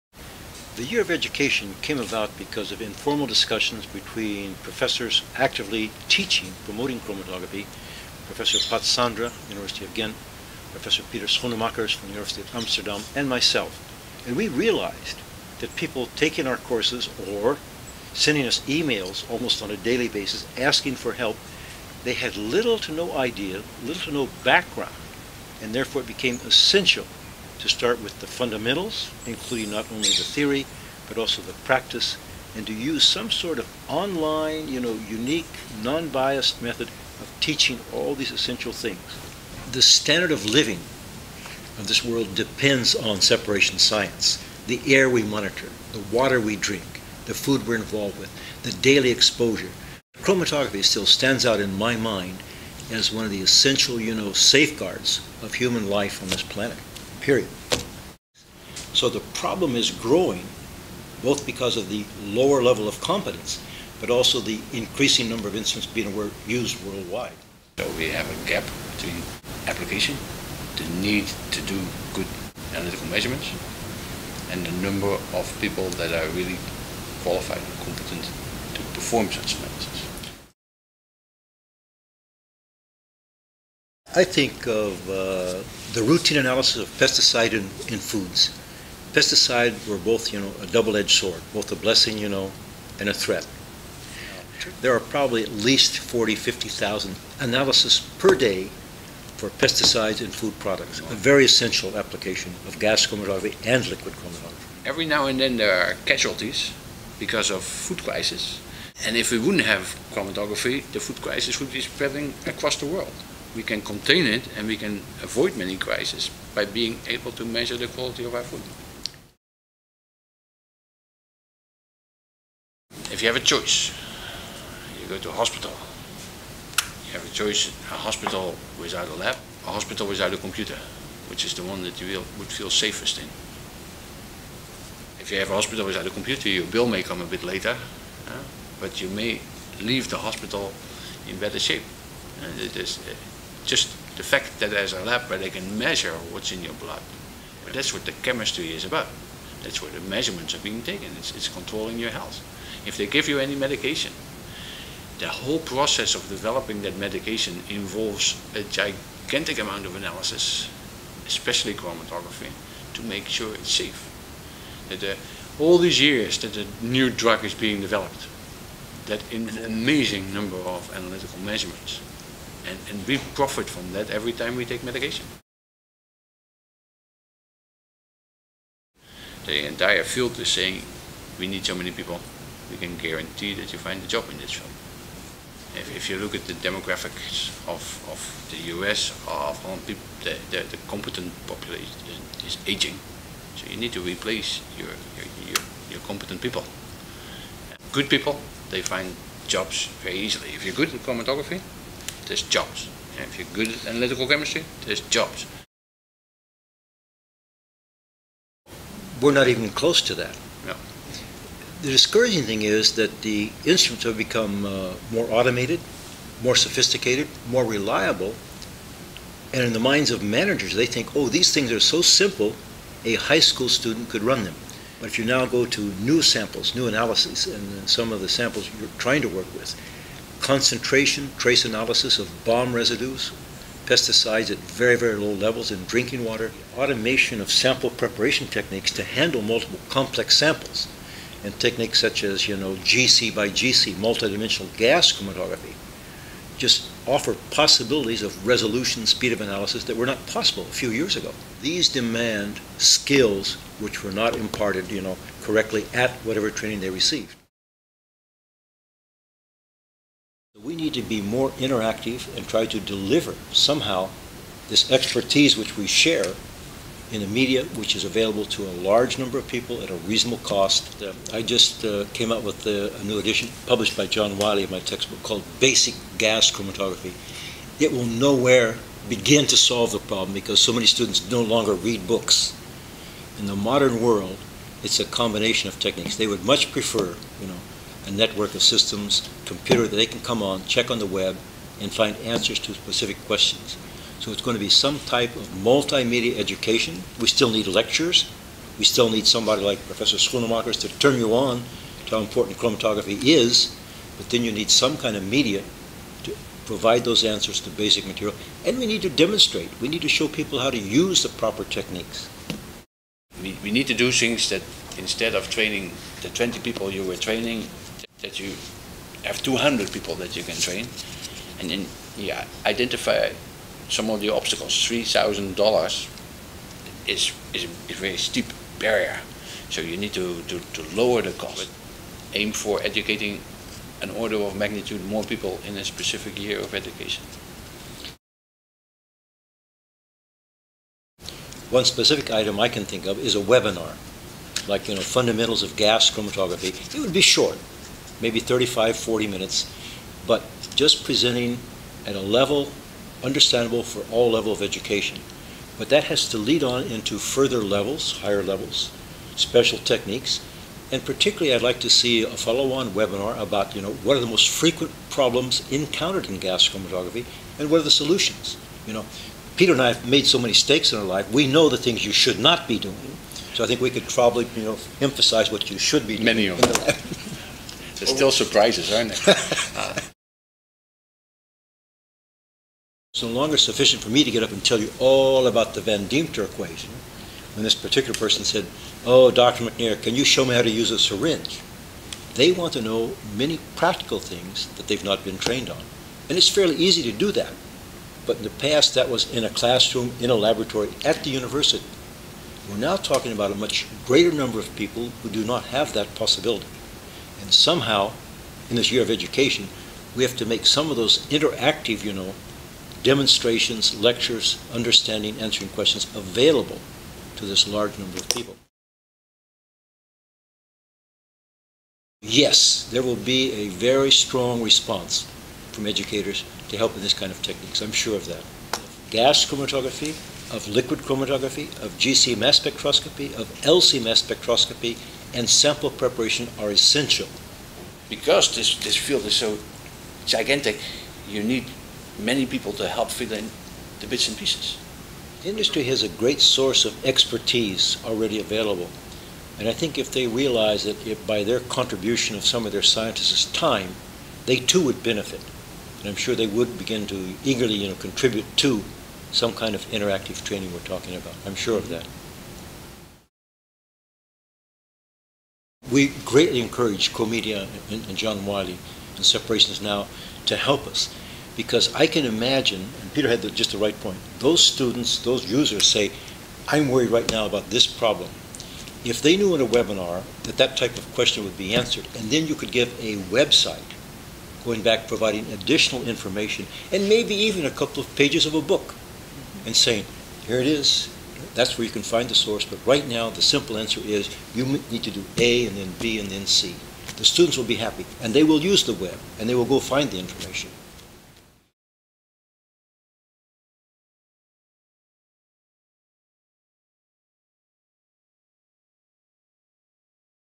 Introducing the "Year of Education" in separation science
Harold McNair, Virginia Tech, Blacksburg, and Peter Schoenmakers, University of Amsterdam, explain the urgent need to improve training and education in chromatography. They explain the importance of chromatography in human life on this planet, our environment, health, safety and food and propose a plan to boost chromatography training by using multi-media, in cooperation with many experts in the field and supported by the online publications separationsNOW.com (a Wiley online initiative) an online source of tutorials and know-how.